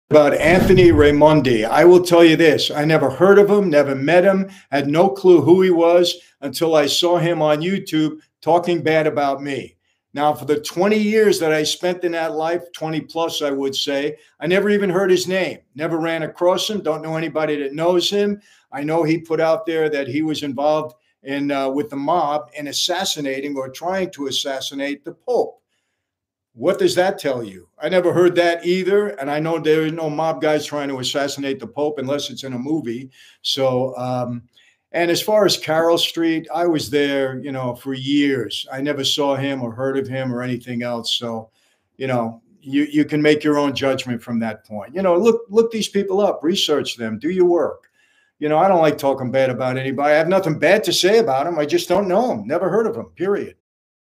Michael Franzese Talks About NOT KNOWING Anthony Raimondi 🤯mafia
mafia crime Into the realm of organized crime with former mobster Michael Franzese as he unveils intriguing anecdotes about his past encounters—or rather, the lack thereof—with Anthony Raimondi. In this captivating YouTube short, Franzese, renowned for his insights into the inner workings of the mafia, offers viewers a rare glimpse into his personal experiences. Discover Franzese's candid reflections on the enigmatic figure of Raimondi and the dynamics of the underworld during his time. From tense confrontations to unexpected twists, this snippet promises to shed light on a lesser-known aspect of the mafia lore. Join us on this riveting journey through the shadows of history as we unravel the mysteries of Franzese's past. Hit play now and immerse yourself in the secrets of the mafia world!